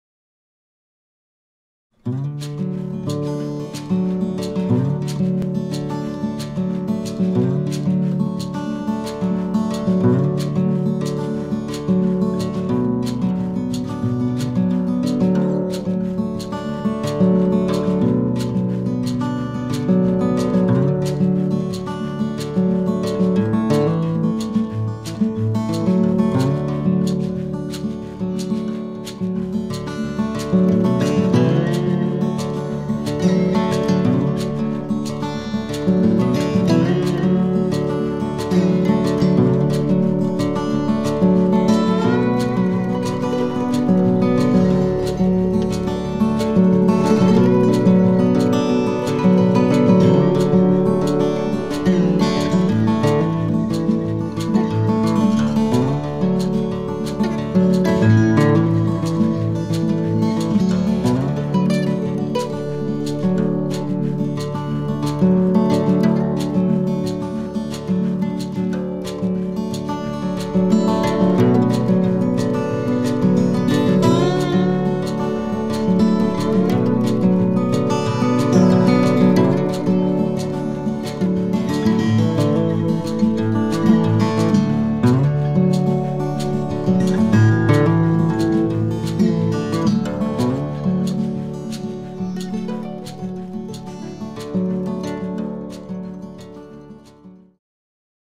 Kot na wypasie.
Kocur szarżuje w karmniku dla ptaków. Degustuje słoninkę wywieszoną dla sikorek.